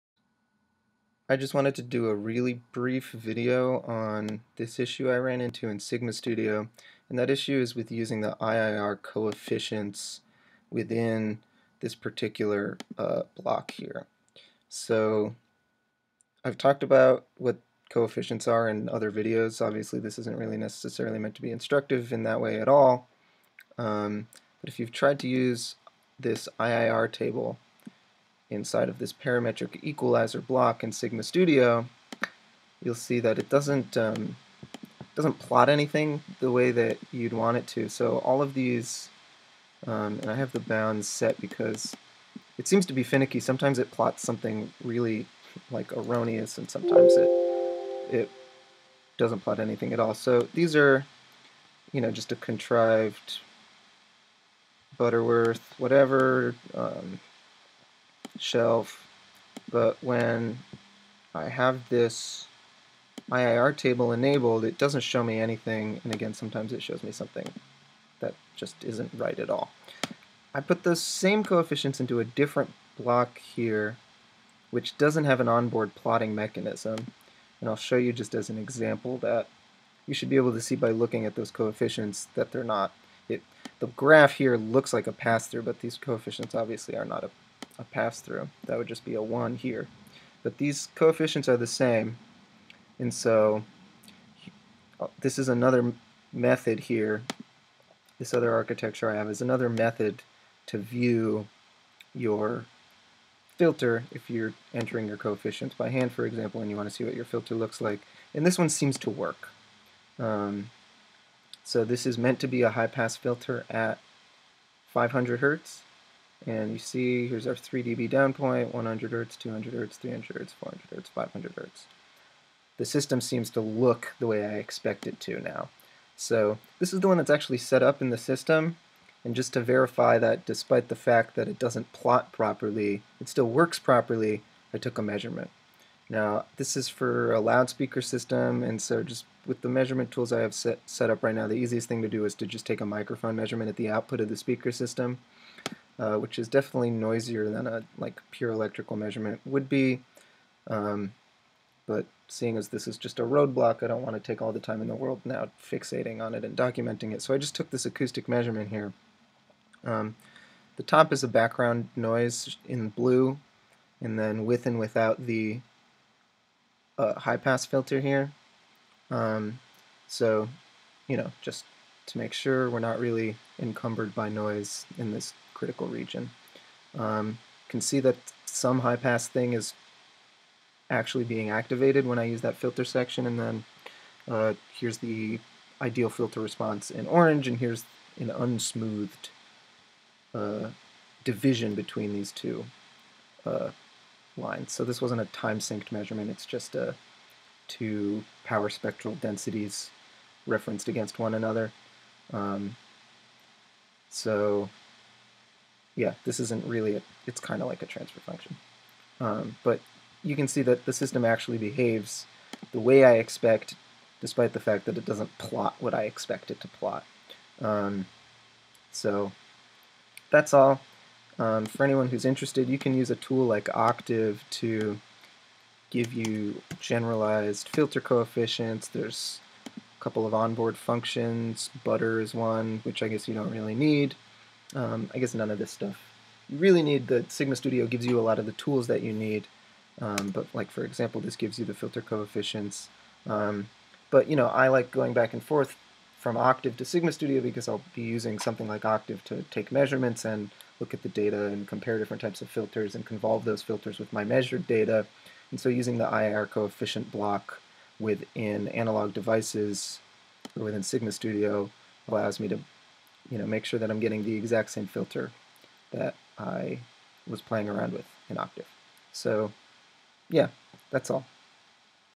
Filter Plotting in Sigma Studio
Solution and verification of issue plotting filters in Sigma Studio using the IIR Coefficient Table within the Parametric Equalizer block